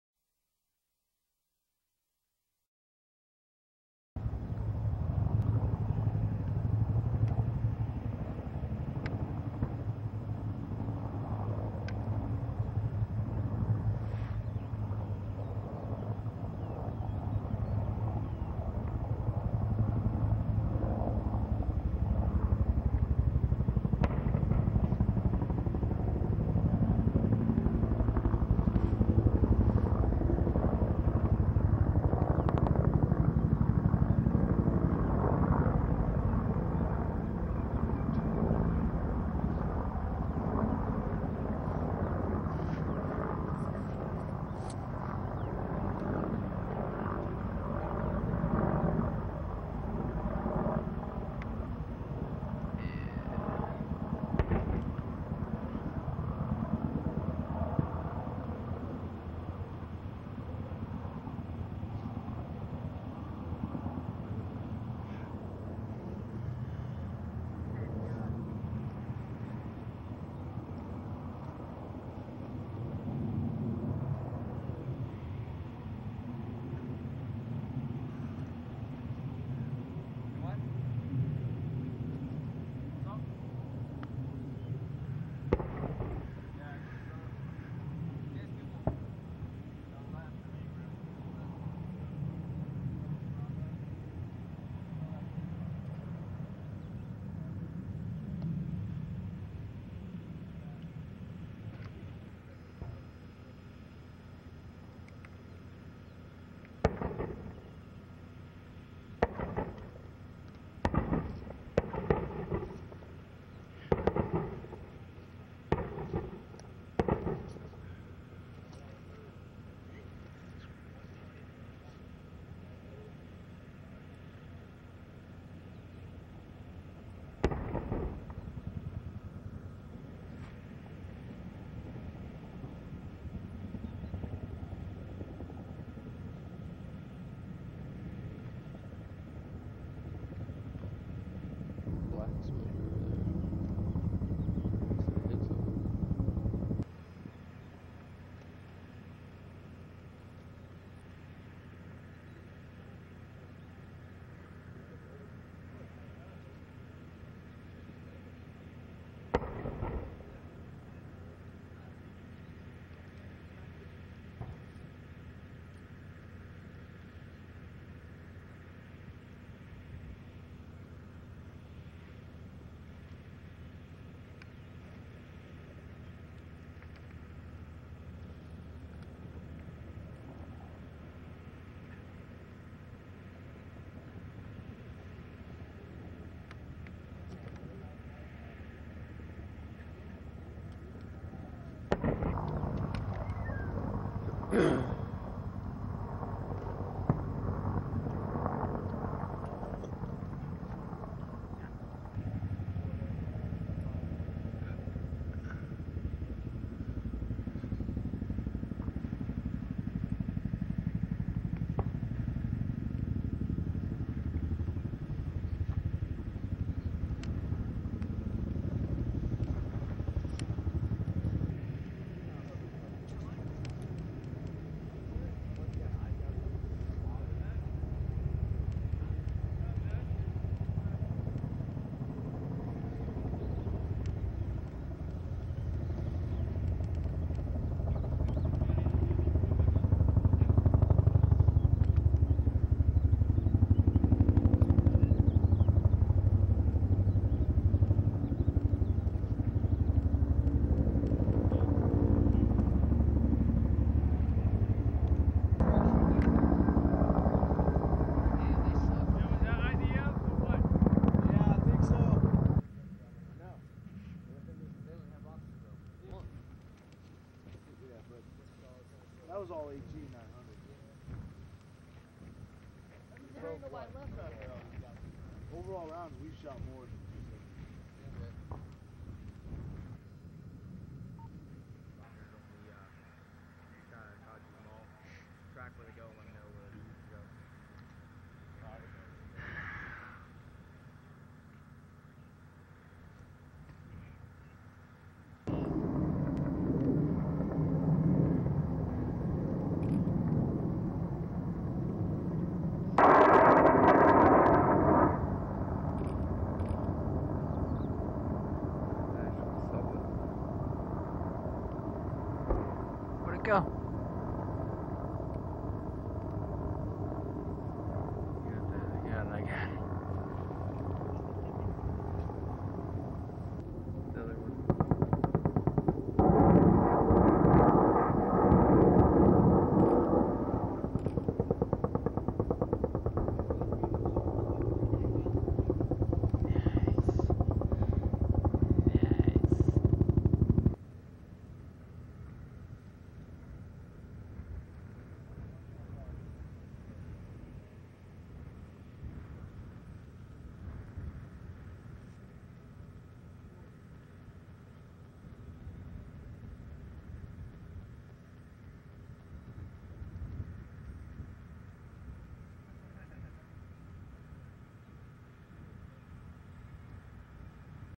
Pounding the Ground in Afghanistan
Video by Lance Cpl. Paul Miller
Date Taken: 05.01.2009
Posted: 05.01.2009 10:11
FOB Now Zad, AF

B-roll of U.S. Marines of Company L, 3rd Battalion, 8th Marines (Reinforced) conducting Operation Eastern Resolve from Forward Operating Base Now Zad, Helmand Province, Afghanistan. Scenes include explosives being detonated and smoke rising from several explosions.